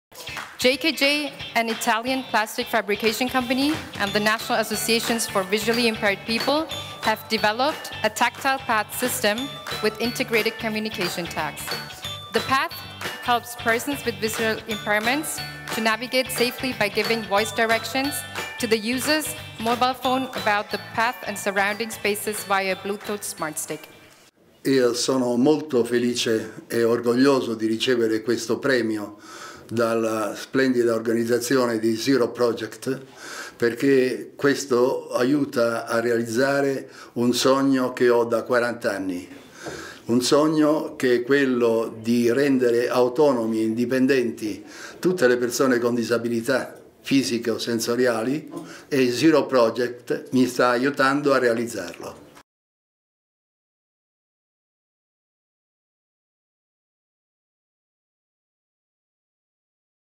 Plastic Solutions: Tactile paths giving voice commands via a smart stick and a mobile phone app
Welcome to the awardee video of LVE smart stick system, a winner of the 2018 Zero Project Award on Accessibility.

JKJ Srl, an Italian plastic fabrication company and Associazione Disabili Visivi Onlus (National Associations for Visually Impaired People) have developed the Loges Vet Evolution (LVE) tactile path system with integrated communication tags. The path helps persons with visual impairments to navigate safely by giving voice directions to the user’s mobile phone about the path and surrounding spaces via a Bluetooth “Smart Stick.” Over 450 areas (mostly in Italy) have had the technology installed plus two paths in Belgium and one in Canada.